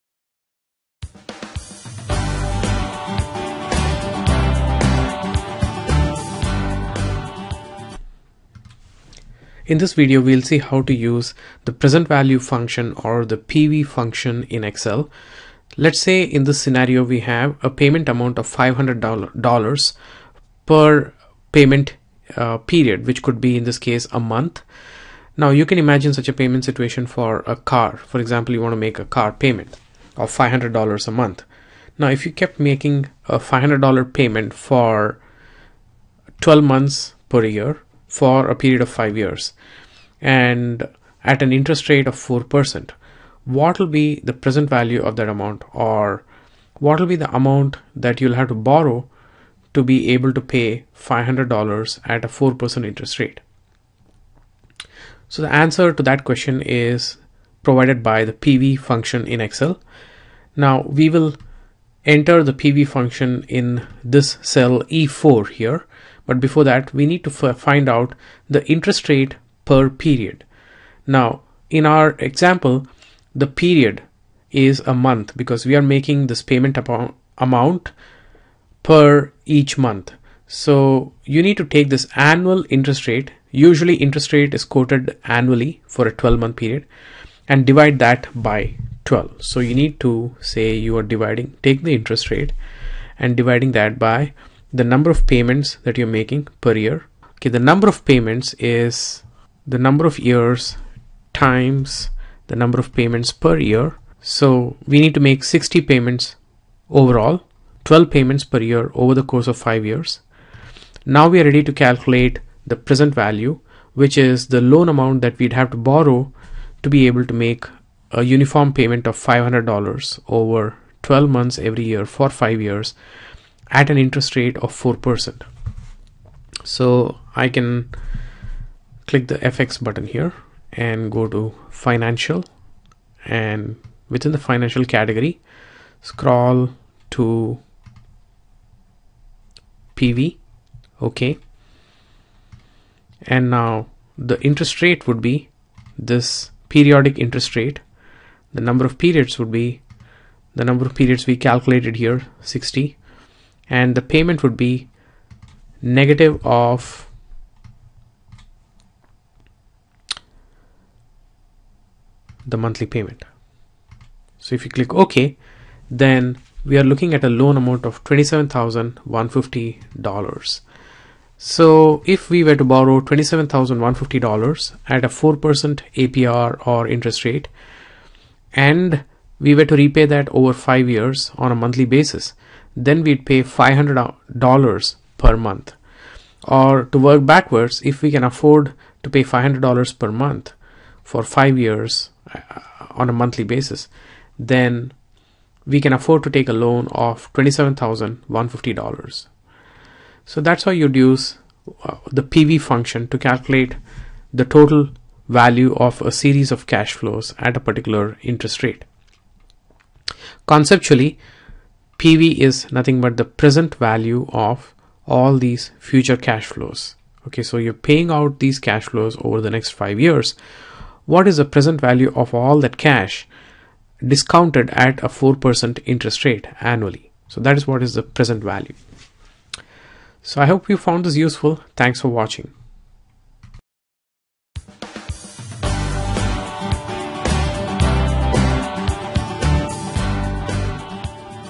Excel tutorial: Present value (PV) in excel 2010
Present value (PV) function lets you calculate the present discounted value of a series of future cash flows. In this example we see how to calculate the loan amount you can borrow for a given series of equal monthly payments like, say a car loan payment.

Some good books on Excel and Finance:

Financial Modeling - by Benninga:

Principles of Finance with Excel - by Benninga: